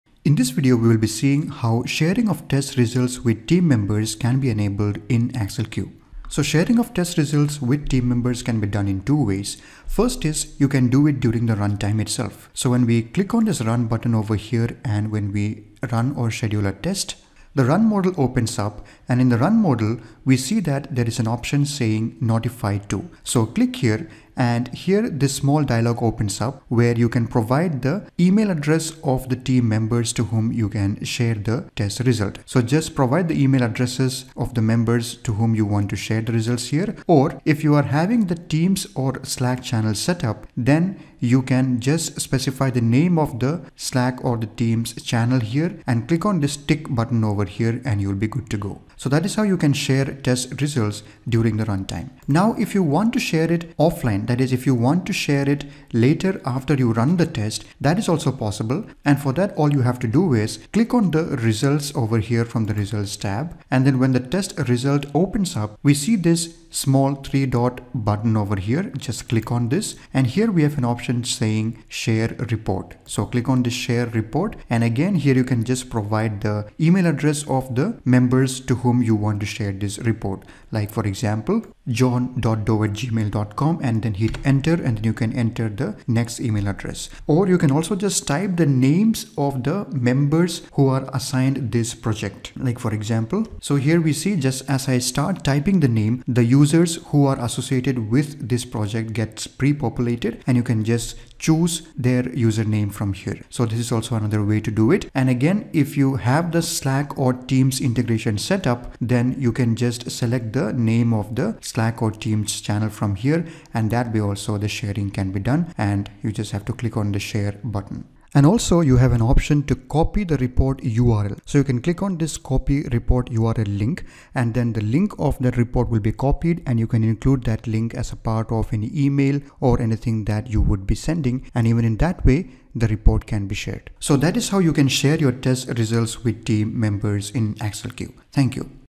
Sharing Test Results with Team Members
ACCELQ Report link along with a summary of test case status can be emailed to a specified list of recipients. You can also export the report in pdf and excel formats for further sharing.

ACCELQ also supports Microsoft Teams integration to notify test execution and results to team members on Teams channels.

You can also set up Slack integration in ACCELQ to notify test execution and results to team members on slack channels.